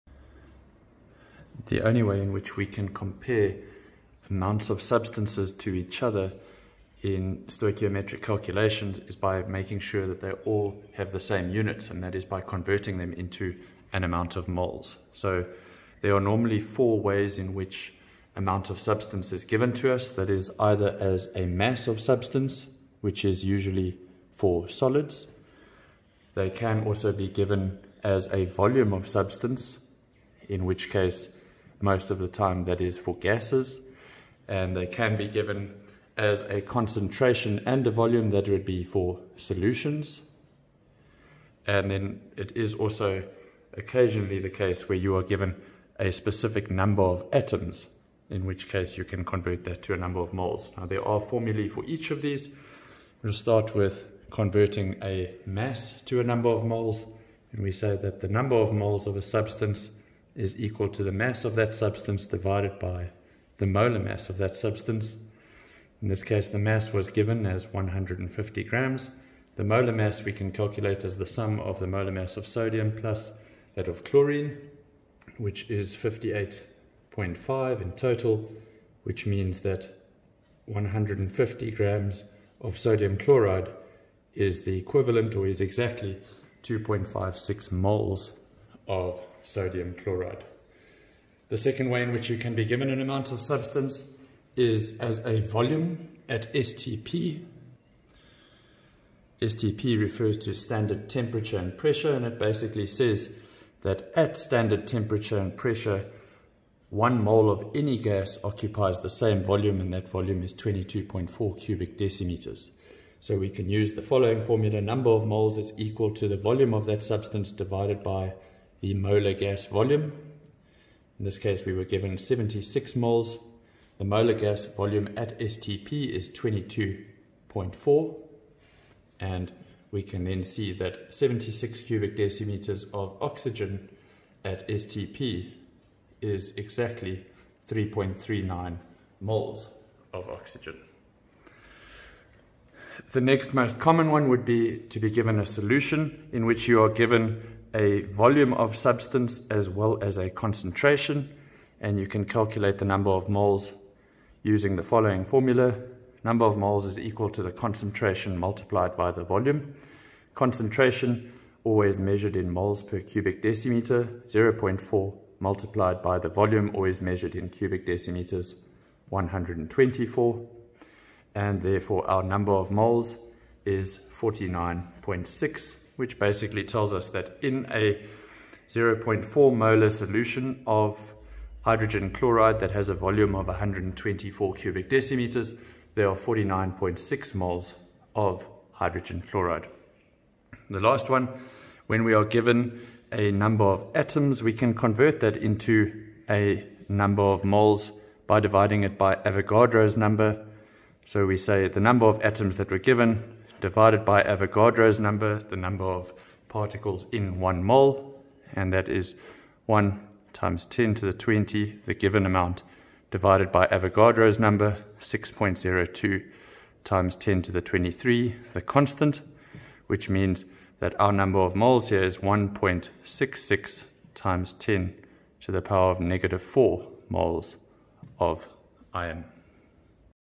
Converting to Moles
This videos how you convert given masses, volumes, concentrations and numbers of particles into a number of moles.

South Africa
CAPS
Stoichiometry